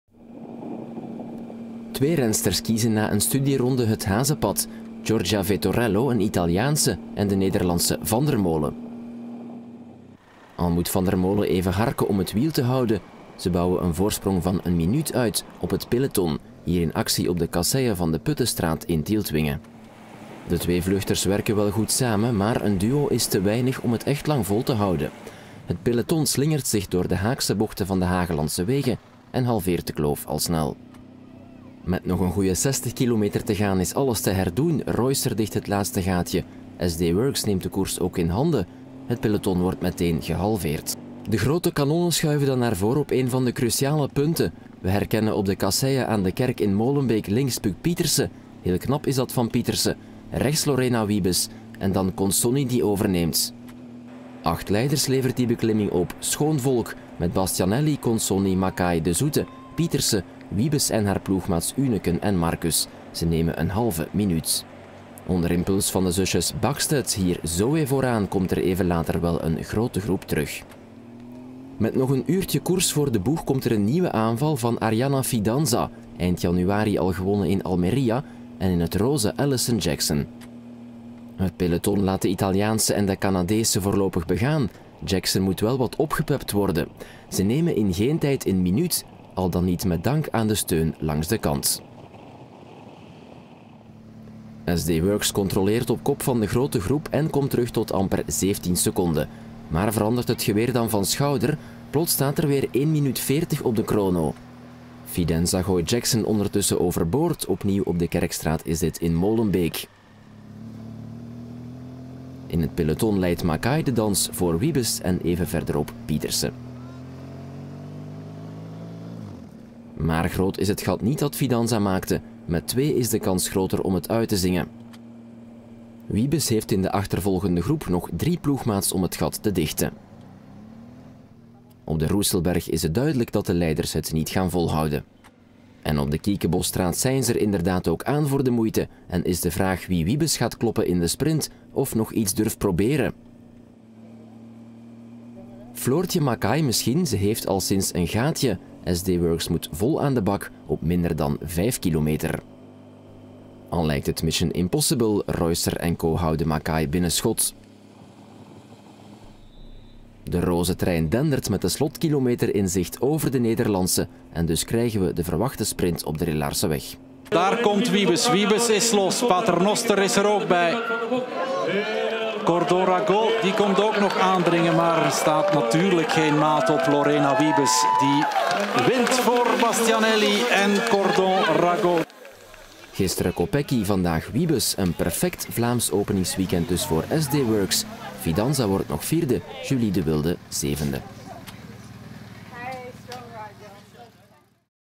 Topfavoriete Lorena Wiebes wint Omloop van het Hageland 2023
Vanmiddag stond de Omloop van het Hageland op het menu. 124 kilometer moesten er afgelegd worden tussen startplaats Aarschot en de finish in Tielt-Winge, met een aantal venijnige kuitenbijters daarbij, de Roeselberg in Tielt-Winge bijvoorbeeld of de Kerkstraat in Lubbeek. Zoals elk jaar kon de lokale organisatie uitpakken met een aantal wereldtoppers aan de start: Lorena Wiebes, Marta Bastianelli, Chiara Consonni, Floortje Mackay, Zoë Backstedt om er maar een aantal te noemen. Wiebes maakte haar status van topfavoriete waar.